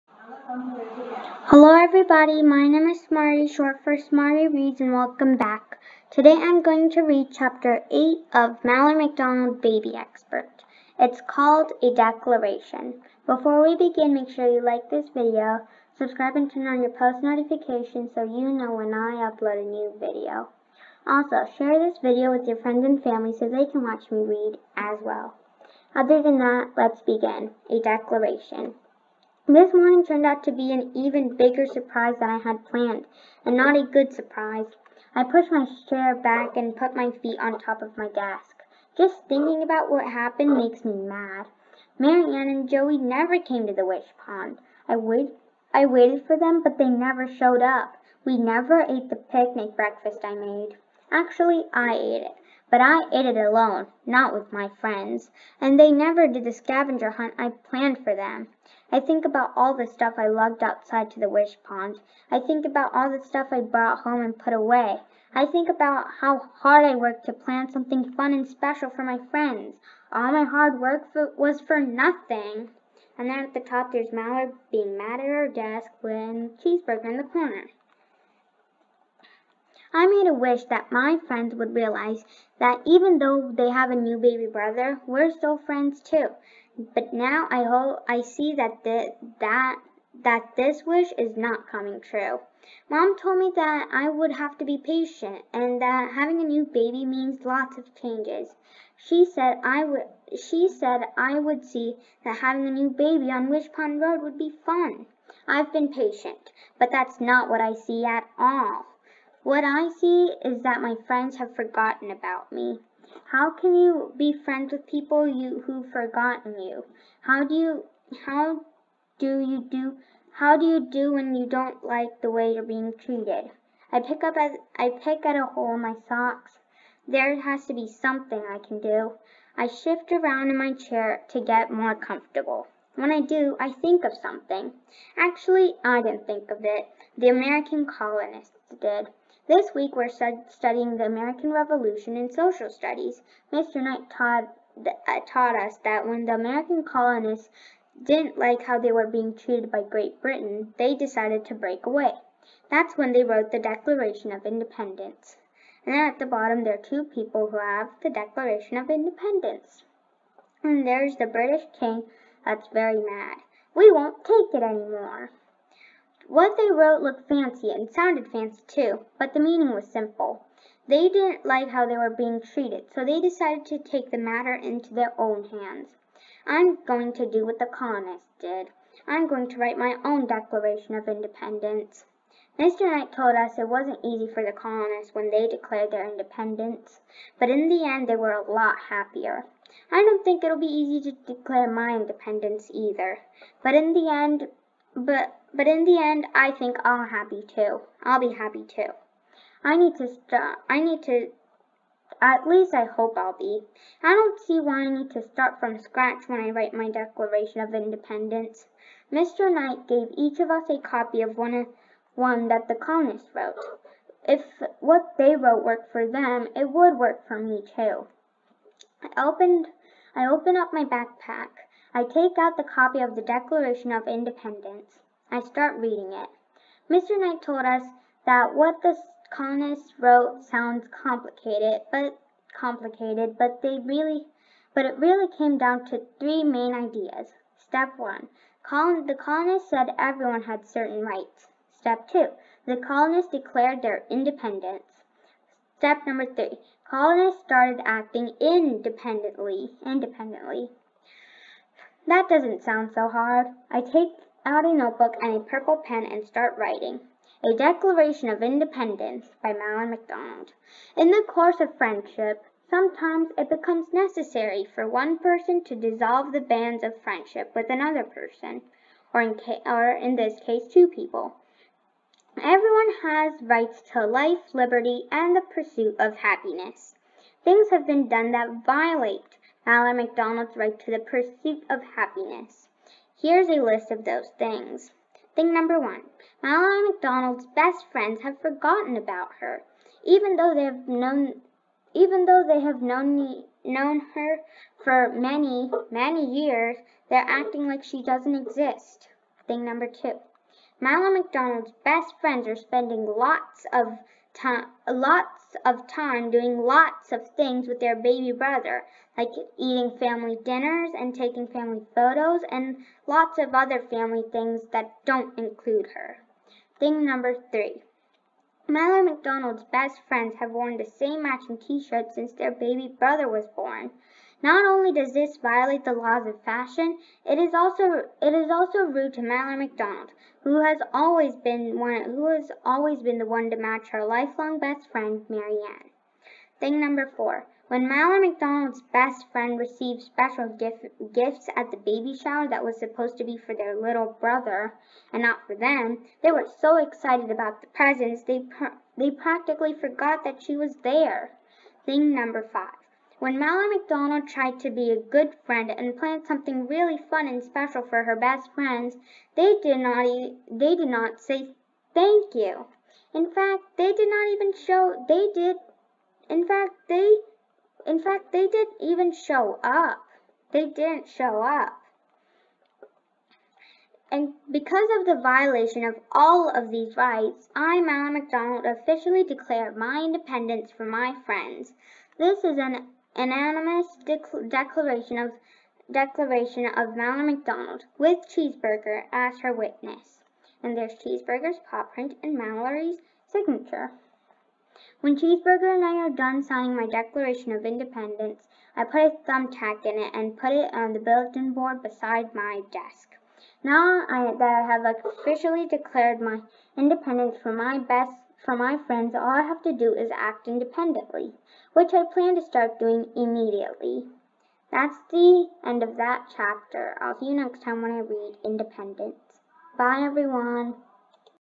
Mallory McDonald, Baby Expert Ch. 9 Reading
Hi everyone! I read chapter 9 of Mallory Mcdonald, Baby Expert. I hope you liked it! Bye! :)